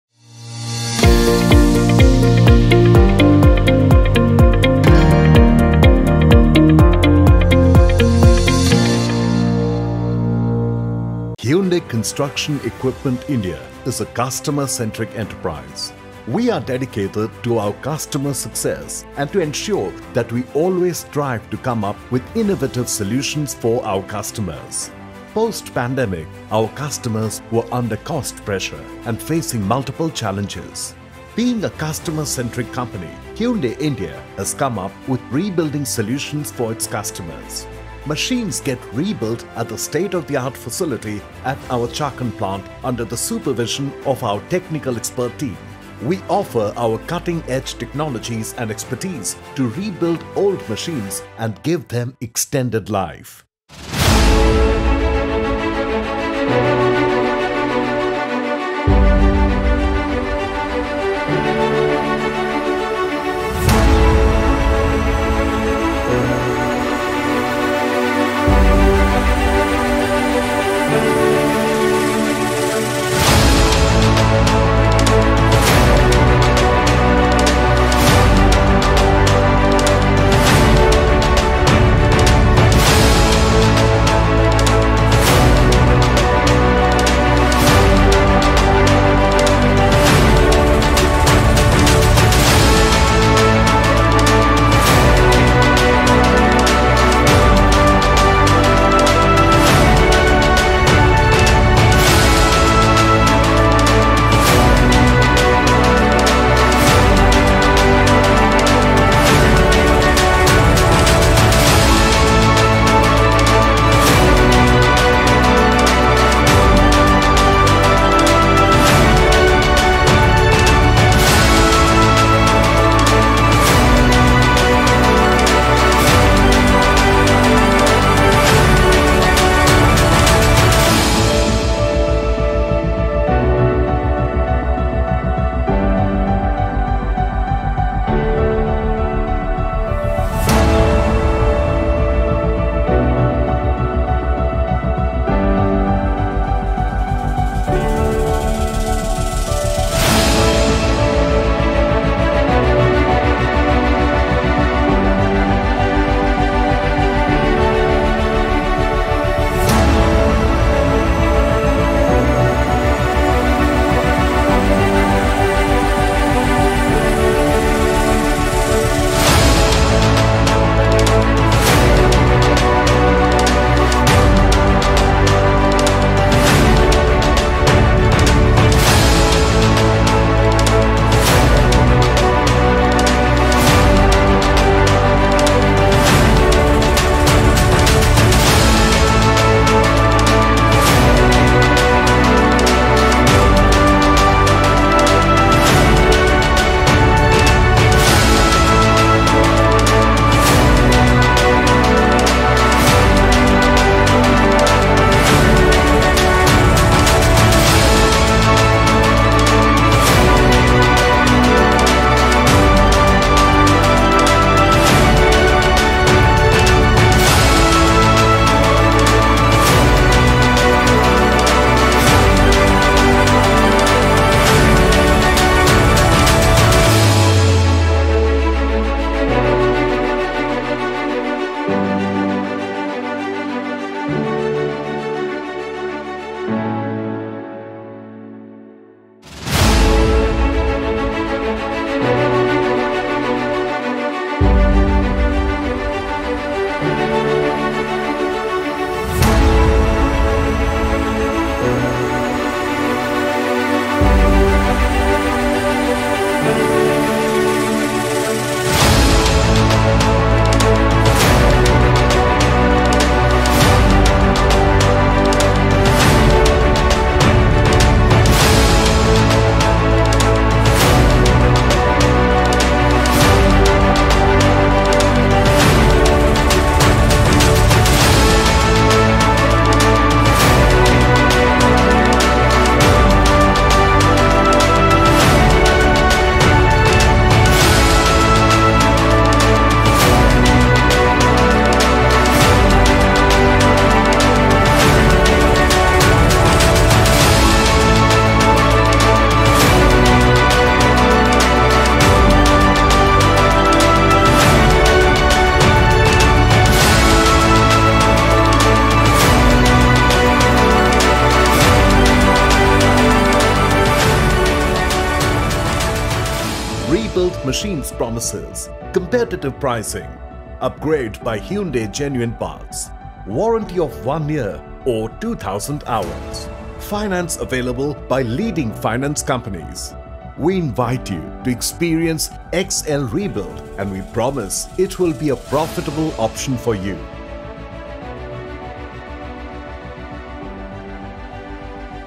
Hyundai Rebuild Solution- The Complete Process
Rebuild simply means to restore something similar to its original condition, and the Rebuilding process is also sometimes called remanufacturing. If we talk about excavators, the rebuilding process consists of replacing and reassembling components to restore the machine to the original state of manufacturing. The first step starts with an inspection of the machine of visible points like visible cracks, leakages, paint, cabin, etc. Then all the major large value components are checked like engine, pump, structure, undercarriage, etc. Generally, the Rebuilding process addresses every component of equipment: everything from basic aesthetic, operator comforts to major components. The ultimate goal is to provide a solution to the customer by producing equipment that is as new as original.
Hyundai Offers two solution under Rebuilding.

1. Rebuild Solution
2. XL Series Excavator